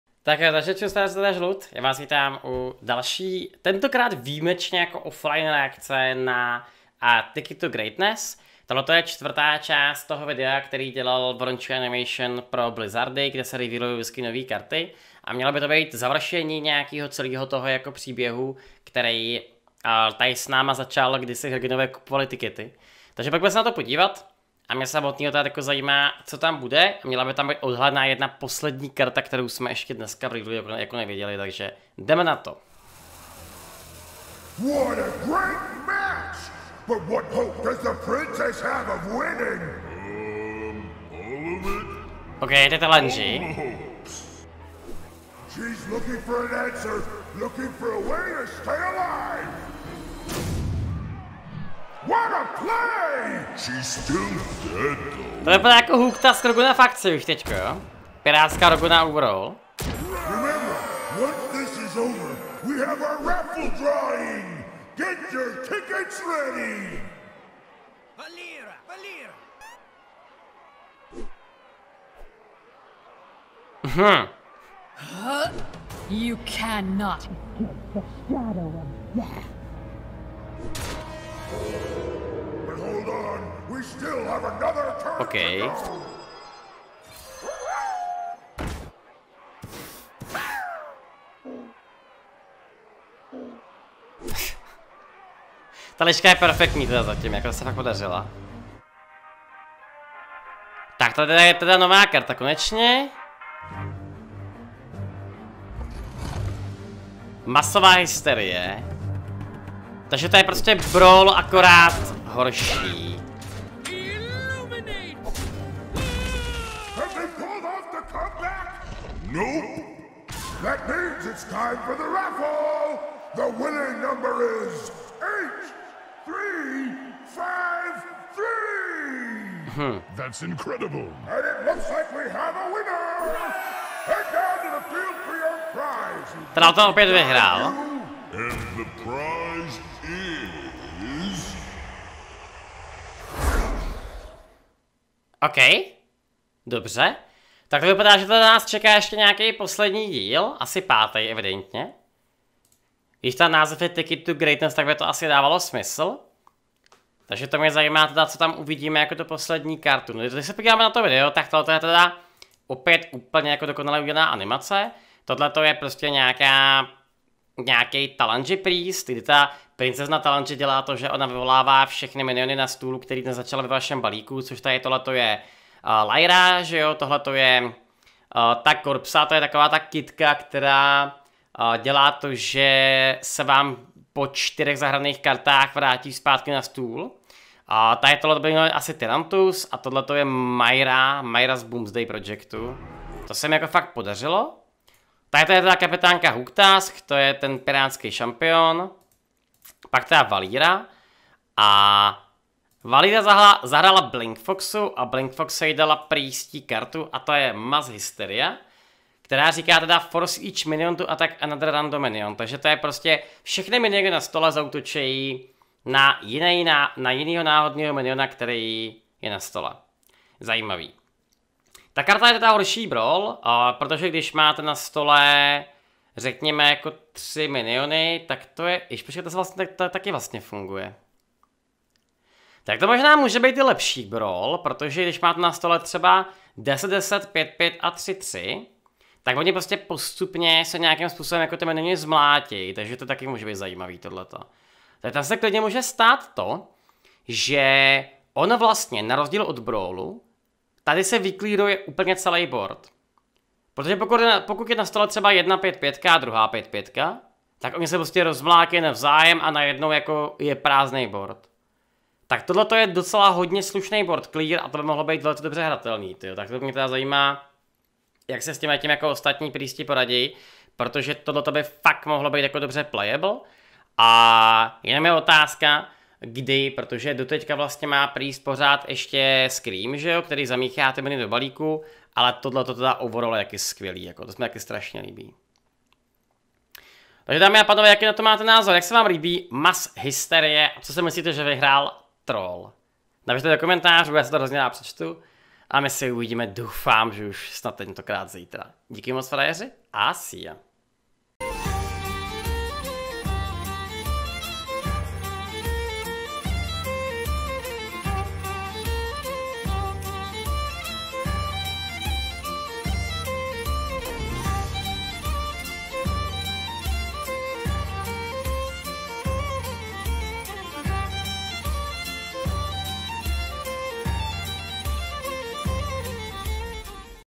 CzechCloud reaguje - Mass Hysteria! Nová karta pro priesta!
Další a možná poslední? Díl Ticket to Greatness je tady a spolu s ním jeden zajímavý zápas a karta pro priesta.